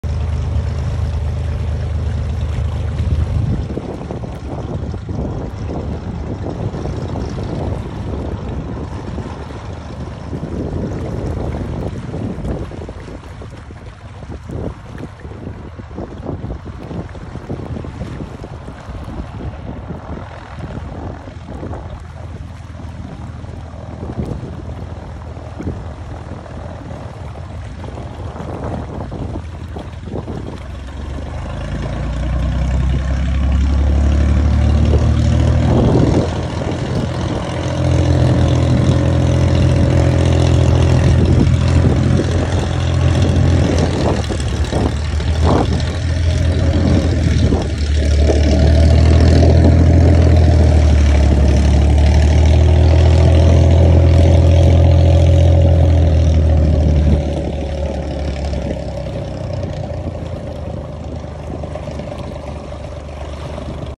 Fishing and watching sea plane
Takes time making round to take off✈️